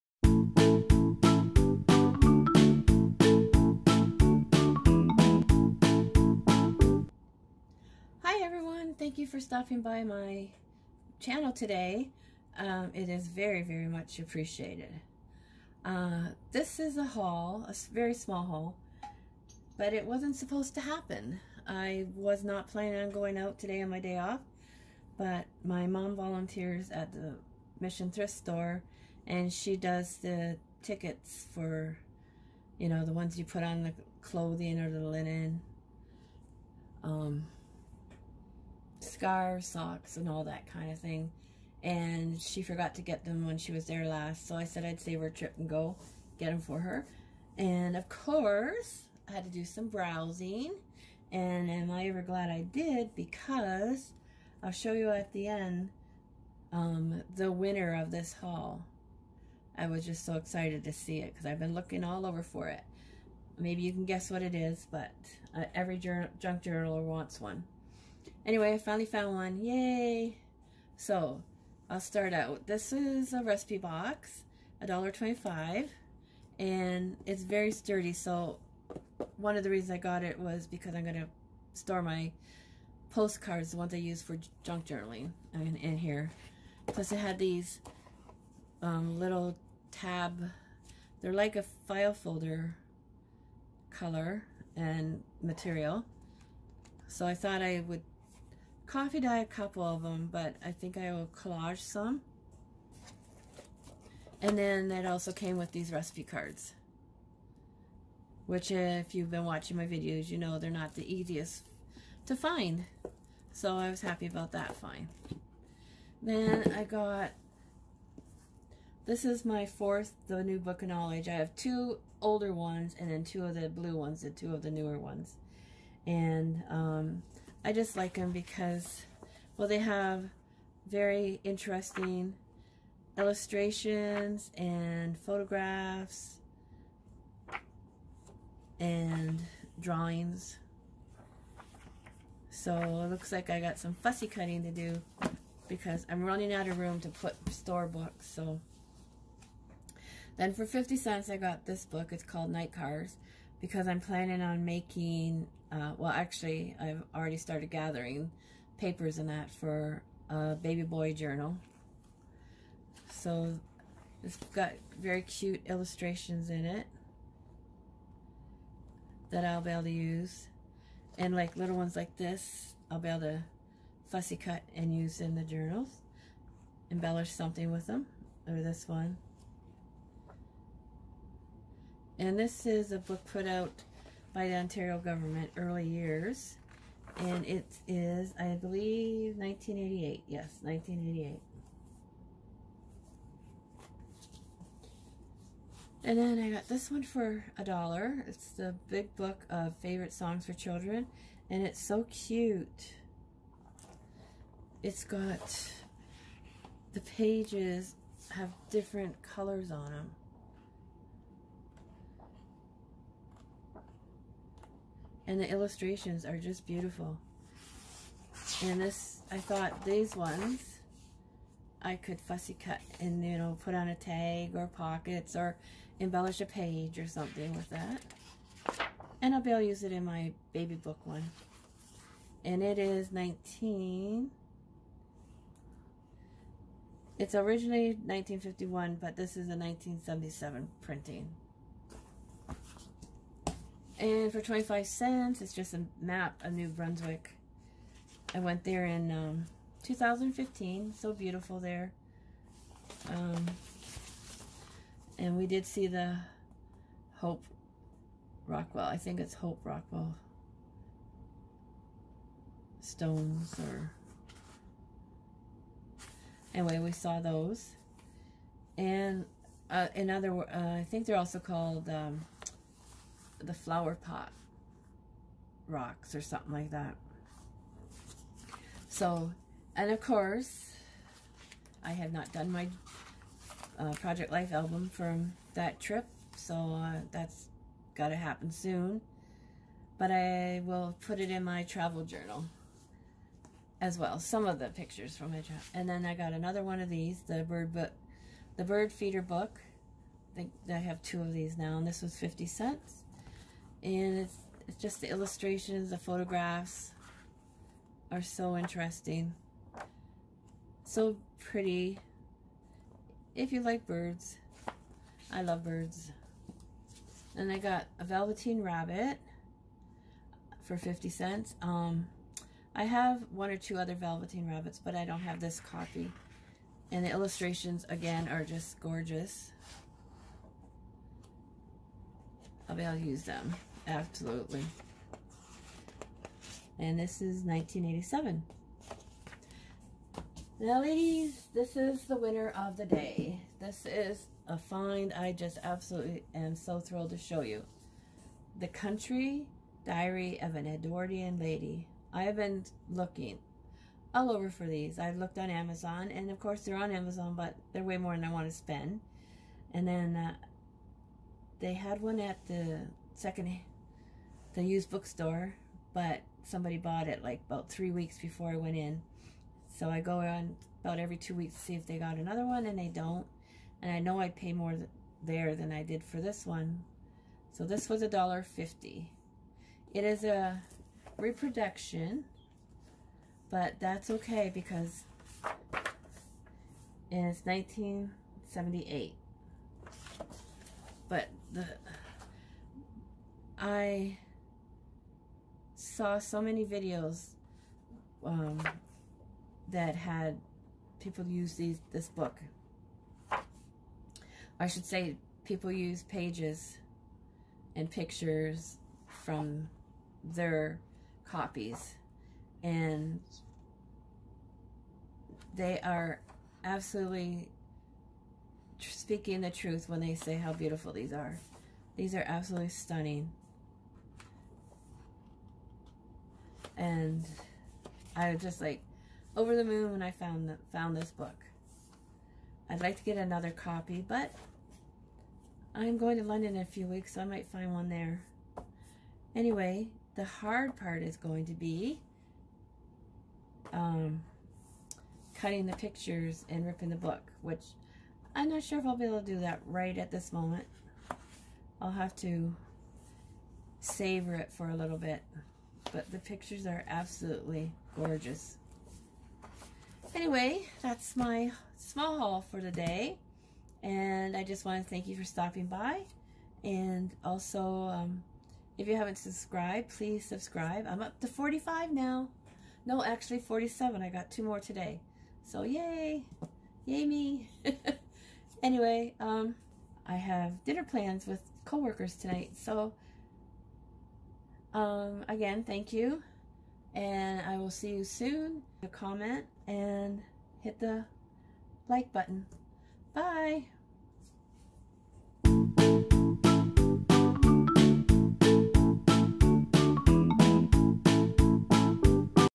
Thrift Store Haul #15 For Junk Journal Supplies
This is a little haul but it has a great find. I was so excited when I saw it.